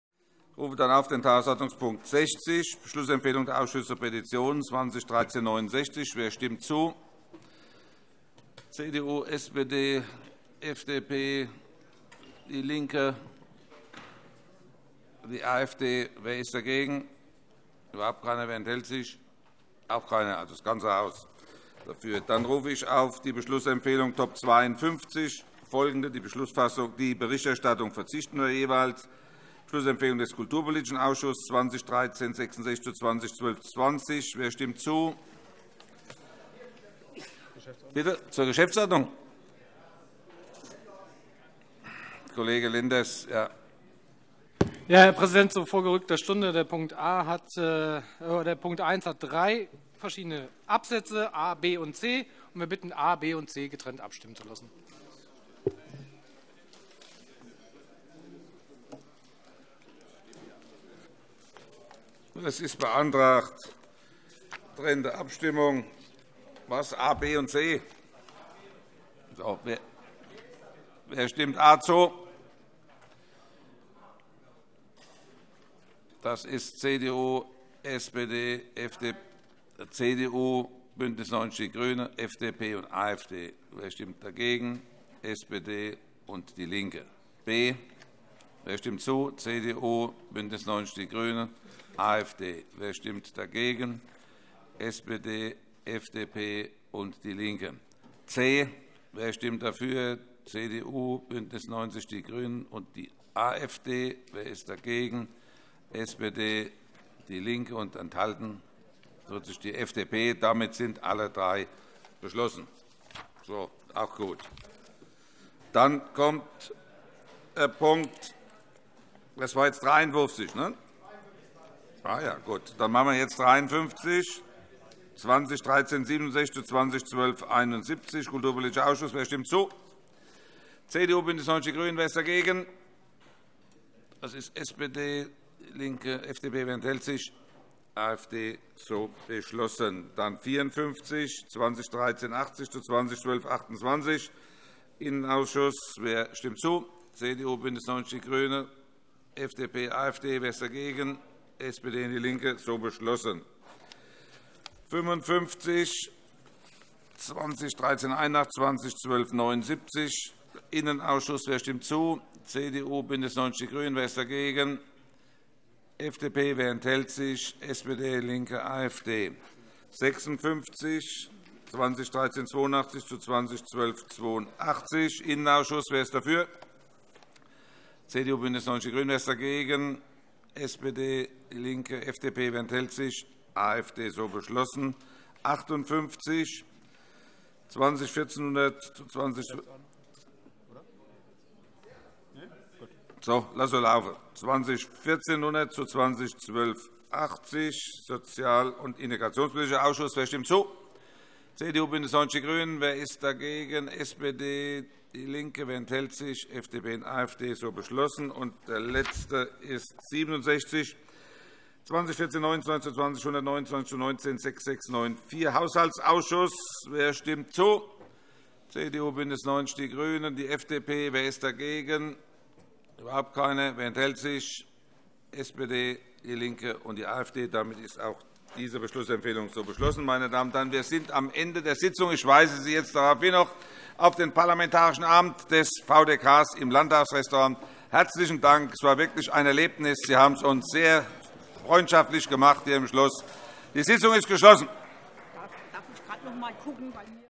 Beschlussempfehlungen - 30.10.2019 - 24. Plenarsitzung
Beschlussempfehlungen der Ausschüsse zu Petitionen

Beschlussempfehlung und Bericht Kulturpolitischer Ausschuss
Antrag Fraktion der SPD
Unterrichtsausfall bekämpfen – mehr Ehrlichkeit statt Verschleierung
– Drucks. 20/1366 zu Drucks. 20/1220

53. Beschlussempfehlung und Bericht
Kulturpolitischer Ausschuss
Dringlicher Antrag Fraktion der Freien Demokraten
Digitale Bildung in Hessen braucht mehr als ein Gesetz zur Umsetzung des Digitalpakts
– Drucks. 20/1367 zu Drucks. 20/1271

Beschlussempfehlung und Bericht Innenausschuss
Entschließungsantrag Fraktion DIE LINKE
Rechte Drohungen, Rassismus, Gewalt und Terror brauchen Gegenwehr – Zivilgesellschaft, Prävention und Strafverfolgung stärken und Waffenrecht verschärfen
– Drucks. 20/1380 zu Drucks. 20/1228

Dringlicher Entschließungsantrag Fraktion der CDU, Fraktion BÜNDNIS 90/DIE GRÜNEN
Klare Haltung und konsequente Maßnahmen im Kampf gegen Rechtsextremismus –
Aktionsprogramm „Hessen gegen Hetze“
– Drucks. 20/1381 zu Drucks. 20/1279

Freiheitlich-demokratische Grundordnung verteidigen, Konsequenzen aus rechtsextremen
Vorfällen ziehen
– Drucks. 20/1382 zu Drucks. 20/1282

Beschlussempfehlung und Bericht Sozial- und Integrationspolitischer Ausschuss
Dringlicher Antrag Fraktion der CDU, Fraktion BÜNDNIS 90/DIE GRÜNEN
Qualitativ hochwertige Versorgung von psychisch Erkrankten gewährleisten
– Drucks. 20/1400 zu Drucks. 20/1280

Beschlussempfehlung und Bericht Haushaltsausschuss
Vorlage Rechnungshof Präsident des Hessischen Rechnungshofs
Bemerkungen 2017 des Hessischen Rechnungshofs zur Haushalts und Wirtschaftsführung des Landes Hessen (einschließlich Feststellung zur Haushaltsrechnung 2017)
– Drucks. 20/1429 zu Drucks. 20/129 zu Drucks. 19/6694